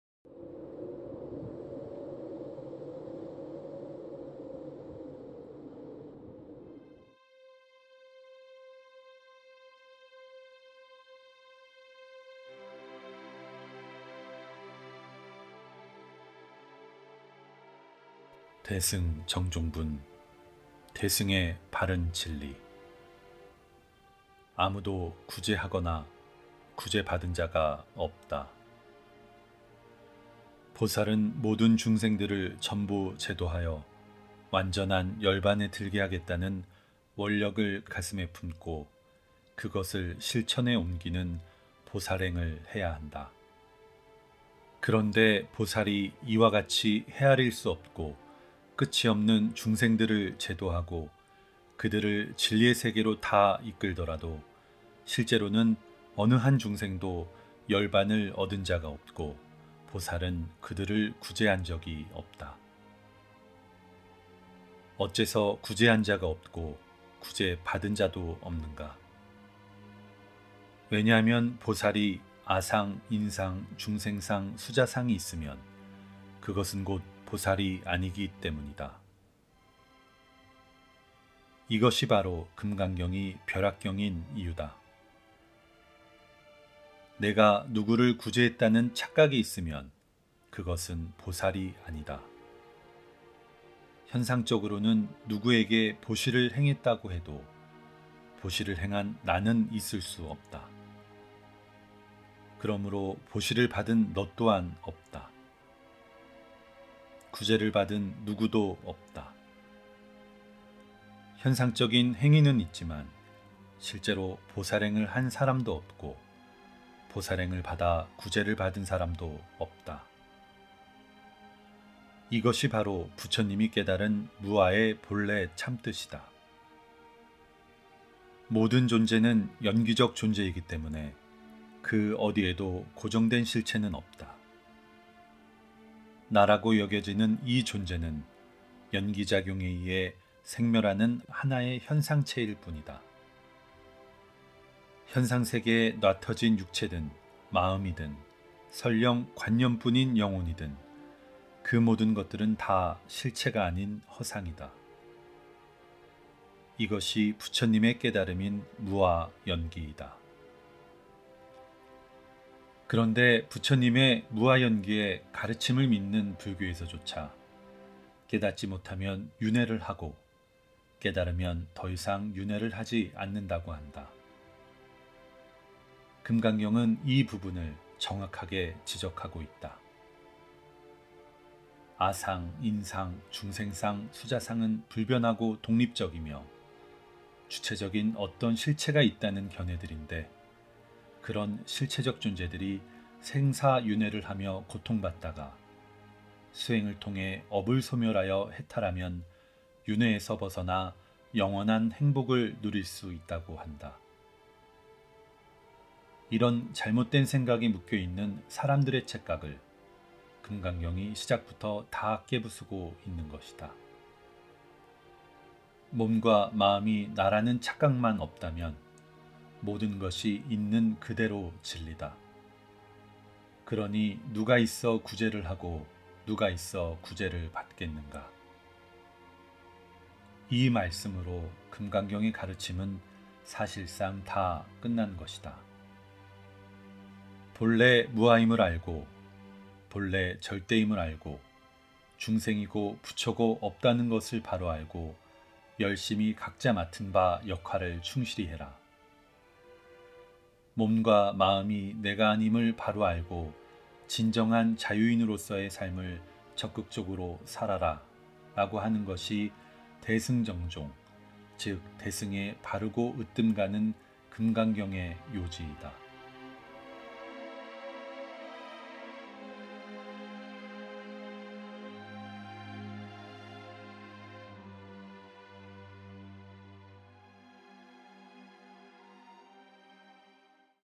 (3회)금강경_대승정종분 大乘正宗分: 대승의 바른 진리 [금강경, 부처님, 무아연기, 명상, 지혜, 깨달음]
금강경:: 꿈속에서 꿈을 깨다

3회_대승정종분 大乘正宗分: 대승의 바른 진리

아무도 구제하거나 구제받은 자가 없다.

도서명: 금강경_무위해공 강설_책과나무